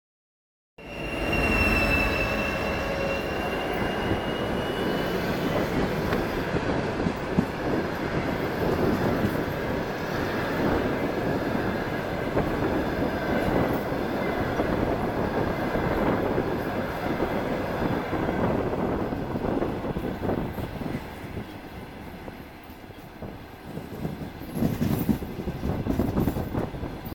ТЕП70-0058 з 100 Новоалексіївка-Мінськ ст.Нова Баварія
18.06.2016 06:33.
ТЭП70-0058 с 100 Новоалексеевка-Минск ст.Новая Бавария
Знято з потягу §124/123 Київ-Костянтинівка.